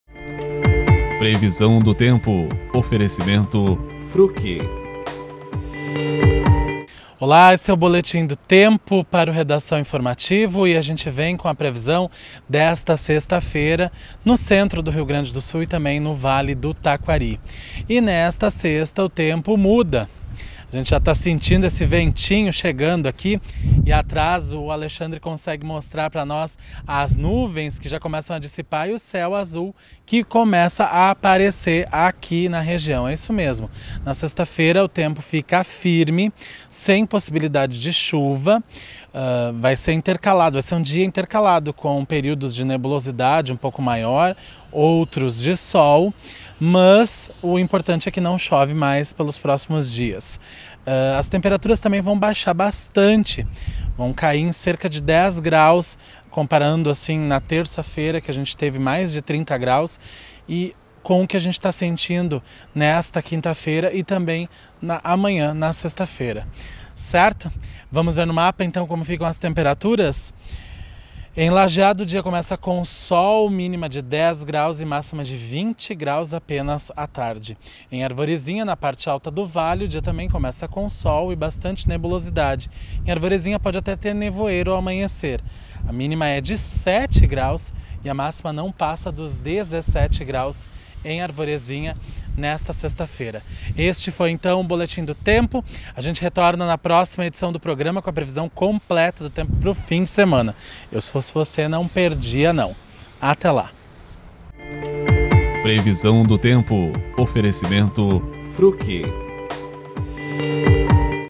Previsão do Tempo 27 10 2016
Confira a previsão do tempo para esta sexta-feira com o jornalista Rodrigo Nascimento.